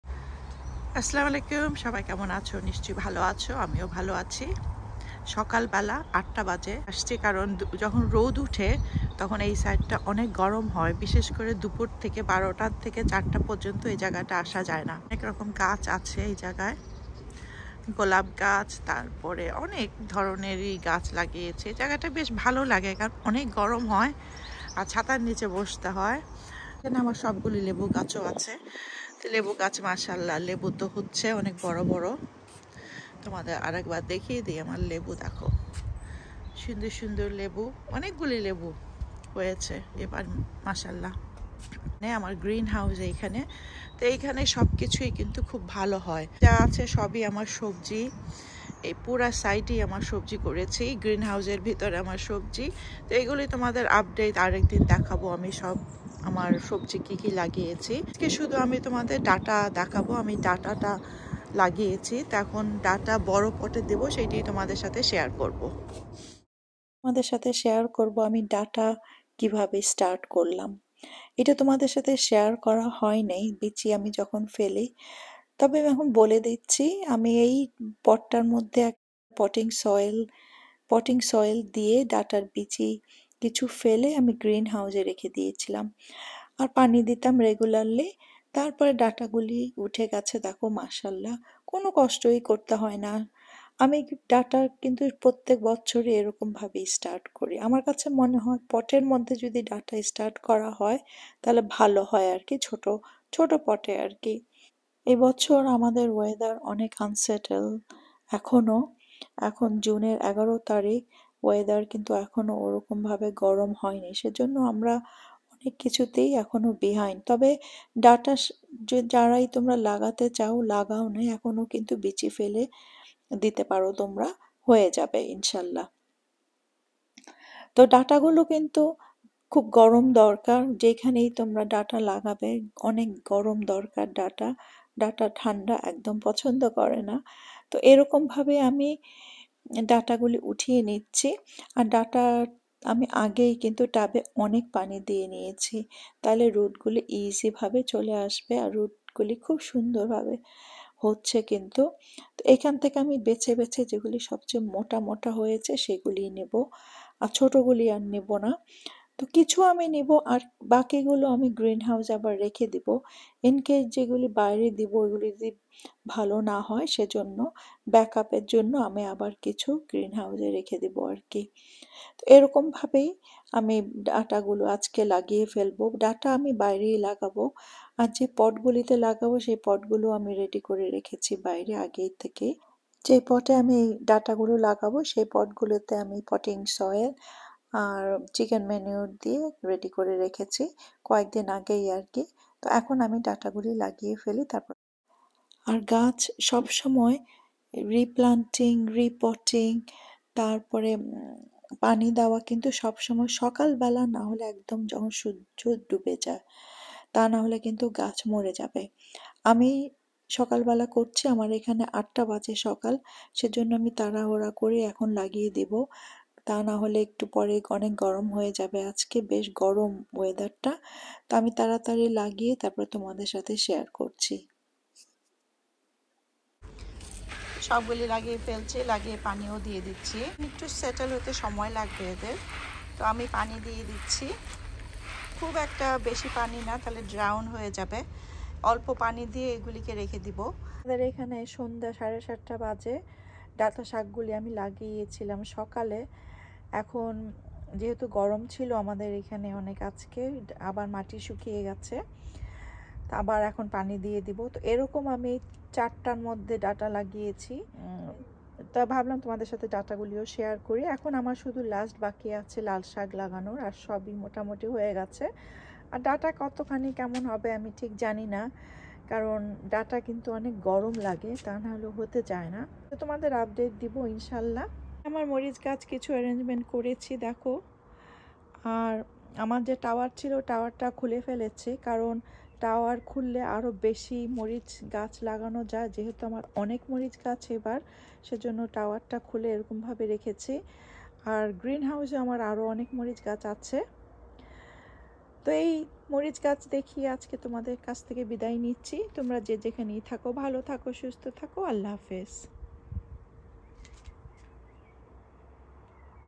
Amar Shokher bagan 2022/ডাটা শাক টবে চাষ করার পদ্ধতি/ Easy way to grow Amarnath seed in uk
How to grow Amaranth in pots easy way. Amaranth can be grown in pots , beds, or in grow bag.